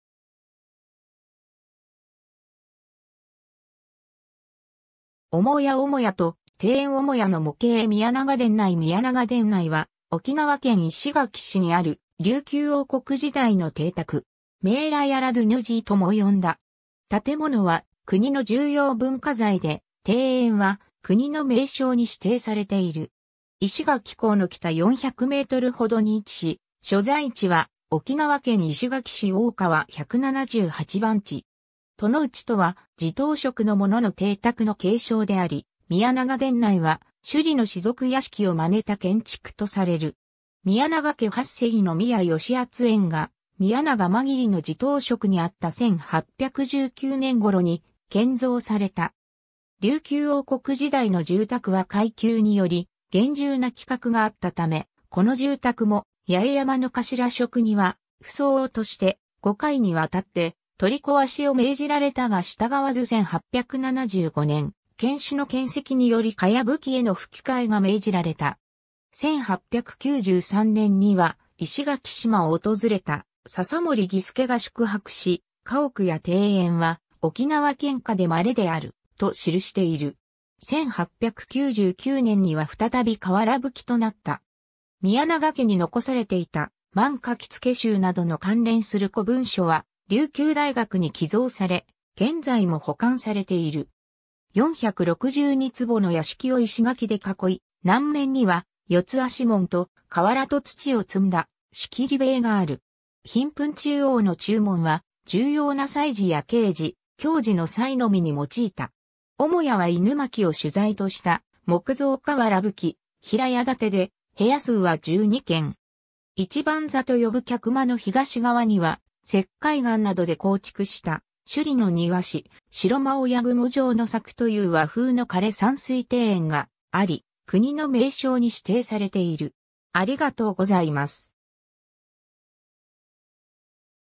宮良殿内
重要文化財_(建造物)
母屋 母屋と庭園 母屋の模型（石垣市立八重山博物館蔵） 宮良殿内（石垣市） 宮良殿内（みやらどぅんち、めーらどうぬじい）は沖縄県石垣市にある琉球王国時代の邸宅。
メーラヤラドゥヌジィとも呼んだ。
建物は国の重要文化財で、庭園は国の名勝に指定されている。
石垣港の北400メートルほどに位置し、所在地は沖縄県石垣市大川178番地。
 殿内とは地頭職の者の邸宅の敬称であり、宮良殿内は首里の士族屋敷をまねた建築とされる。
宮良家8世の宮良当演が宮良間切の地頭職（八重山頭職）にあった1819年（文政2年）頃に建造された。
 琉球王国時代の住宅は階級により厳重な規格があったため、この住宅も八重山の頭職（かしらしょく）には不相応として、5回にわたって取り壊しを命じられたが従わず1875年（明治8年）、検使の譴責により茅葺への葺替えが命じられた。
1893年（明治26年）には石垣島を訪れた笹森儀助が宿泊し、家屋や庭園は沖縄県下で希である、と記している。
1899年（明治32年）には再び瓦葺となった。
宮良家に残されていた『万書付集』などの関連する古文書（『宮良殿内文書』）は、琉球大学に寄贈され、現在も保管されている。
 462坪の屋敷を石垣で囲い、南面には四脚門と、瓦と土を積んだ仕切り屏（ヒンプン）がある。
ヒンプン中央の中門は、重要な祭事や慶事、凶事の際のみに用いた。
 母屋はイヌマキを主材とした木造瓦葺・平屋建で、部屋数は12間。
一番座と呼ぶ客間の東側には、石灰岩などで構築した、首里の庭師・城間親雲上の作という和風の枯山水庭園があり、国の名勝に指定されている。